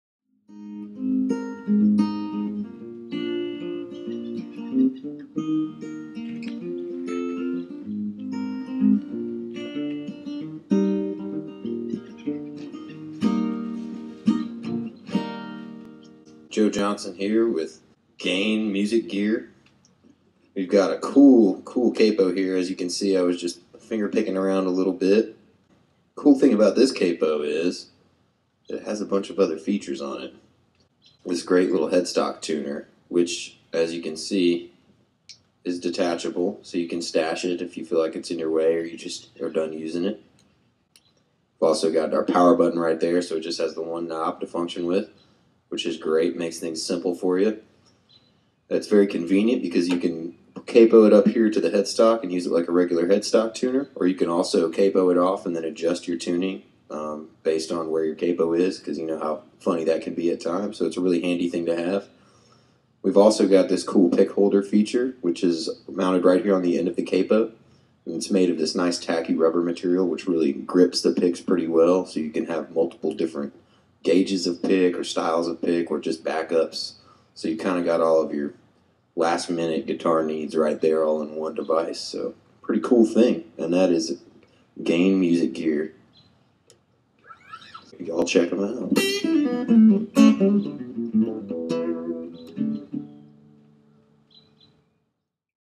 All-In-One Guitar Tuner and Capo and Pick Holder on Acoustic Guitar 1
Joe Jonson here with a demonstration of Guitar Loves All-In-One Tuner and Capo Combo!